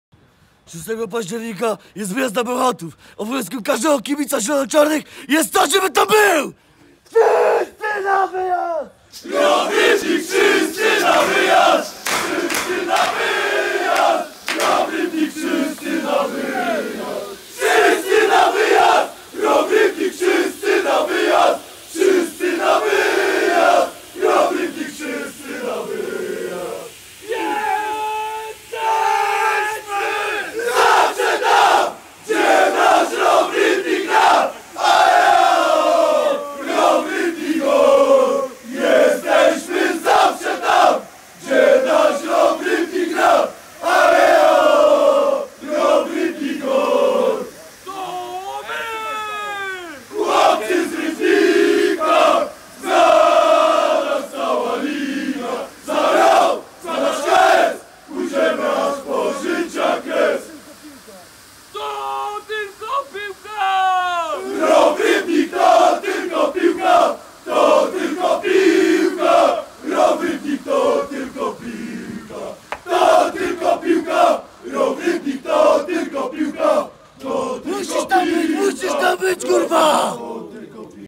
Zapowiedź wyjazdu Bełchatów- ROW Rybnik
Wszyscy jedziemy  na wyjazd do Bełchatowa  06.10.18